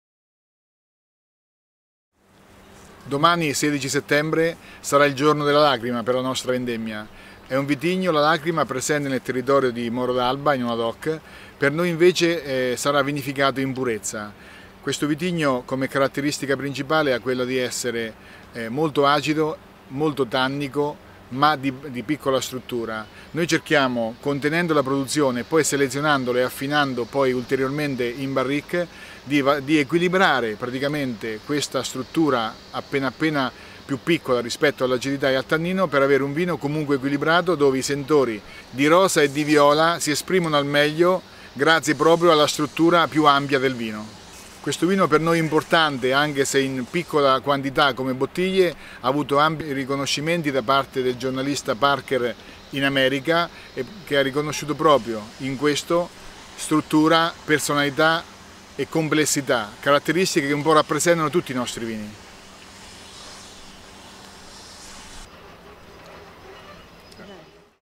Vendemmia Lacrima 16 Settembre 2011
Giovanni Basso, enologo della Cantina Fontezoppa parla del vitigno della Lacrima.
GIovanni Basso, Fontezoppa wine maker, talk about Lacrima